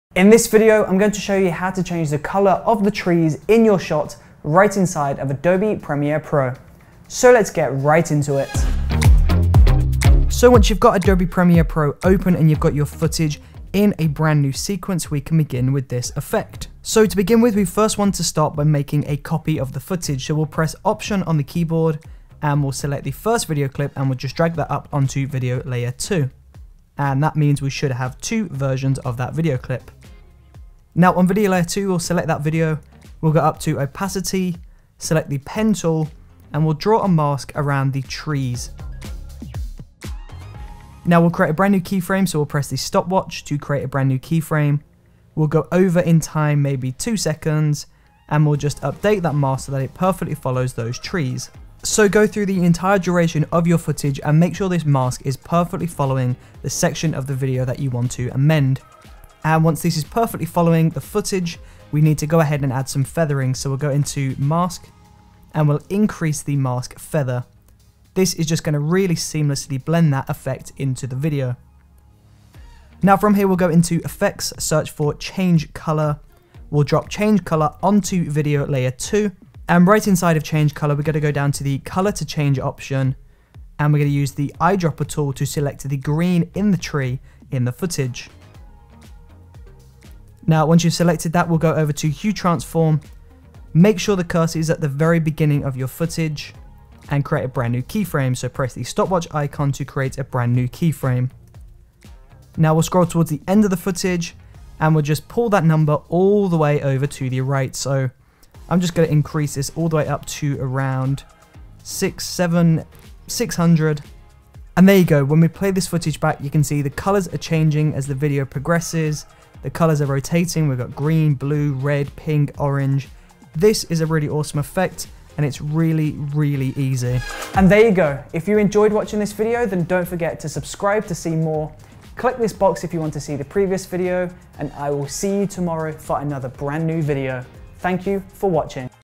Change the Colour of the Trees - Premiere Pro
Use this simple to change the colour of the trees in your footage. You can create some really trippy effects using this, or simply change seasons by turning those green Summer trees to Autumn brown/orange.